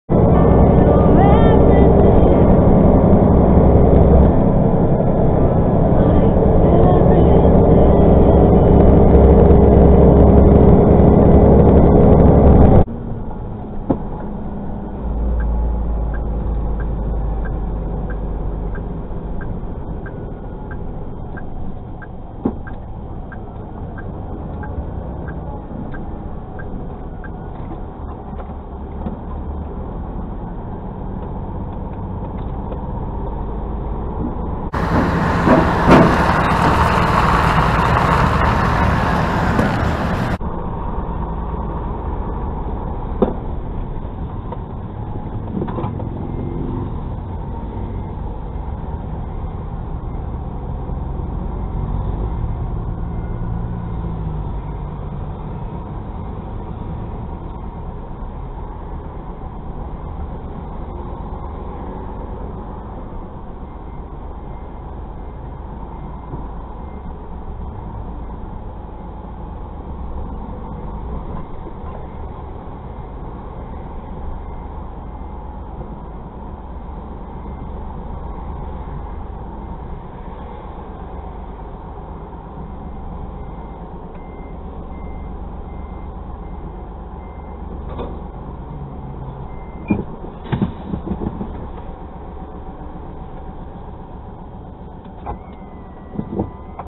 【トレーラー】ＰＡ ちょい左バック【車載カメラ】
神奈川県の西湘バイパスのパーキングエリアで
ちょっぴり左バックです・・・